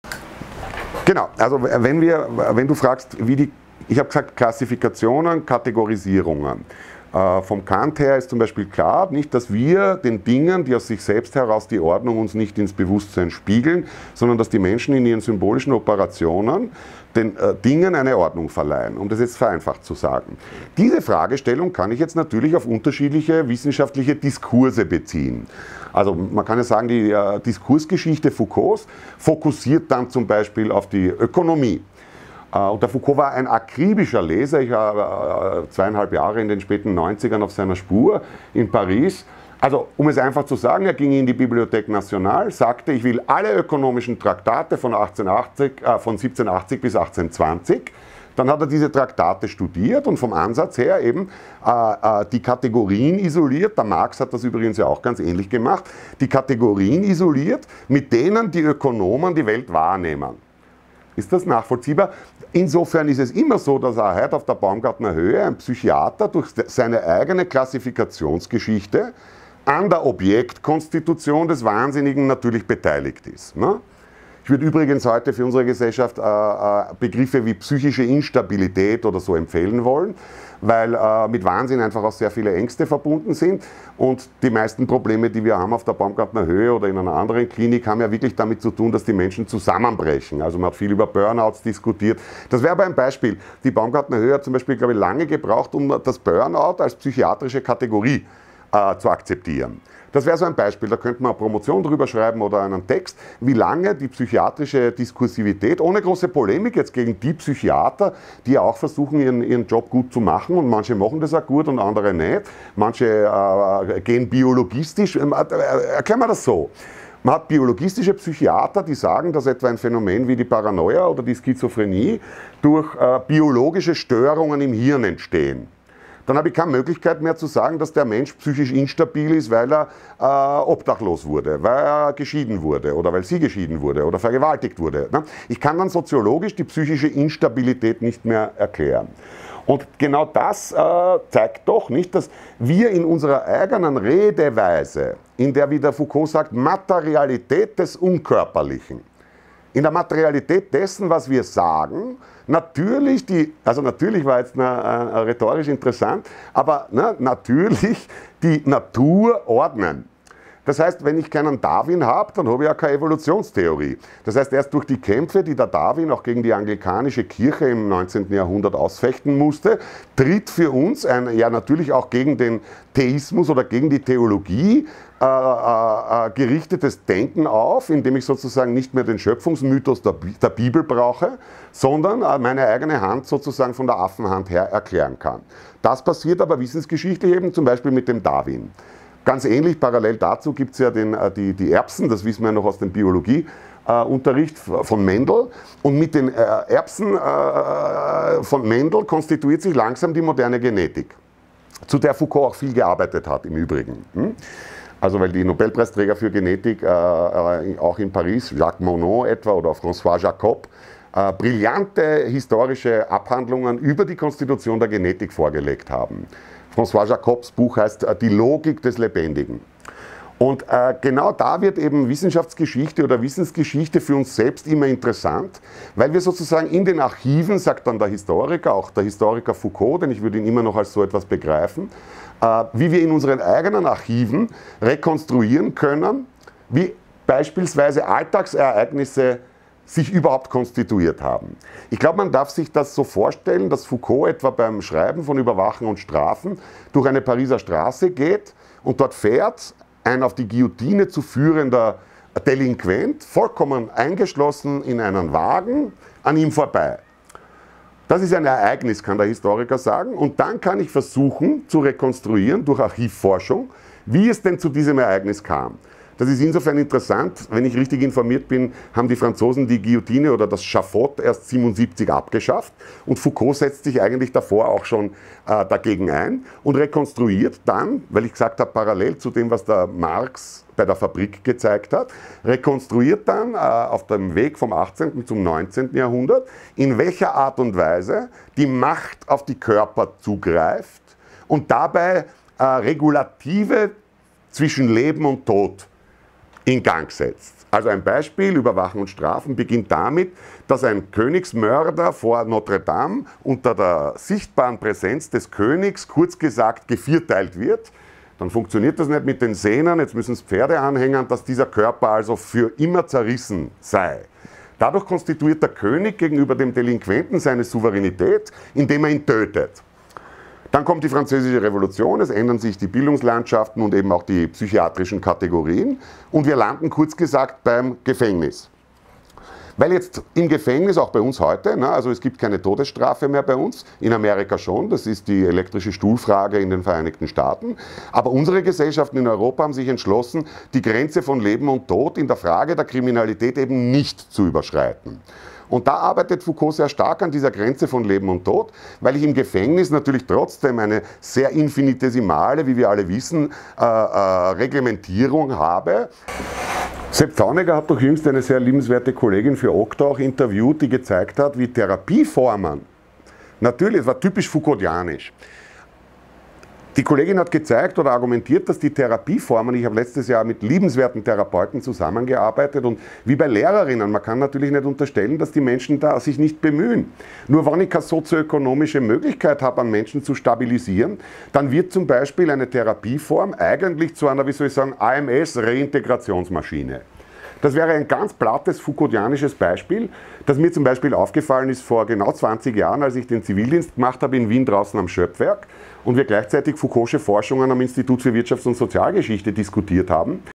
Michel Foucault III: Kategorien und Klassifikationen in verschiedenen Disziplinen
Online course: Seminar zur Diskursanalyse Michel Foucaults und Einführung in sein Werk, Mag. Dr. phil. Alessandro Barberi